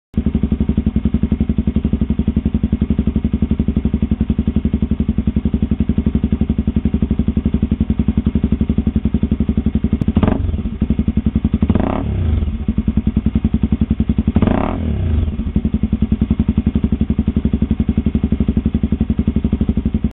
2006 XR250 White Bros. E-Series exhaust sound
XR250のマフラー聞き比べ。お次はホワイトブロス。そこそこヤンチャな音。音量96デジベルくらいで設定。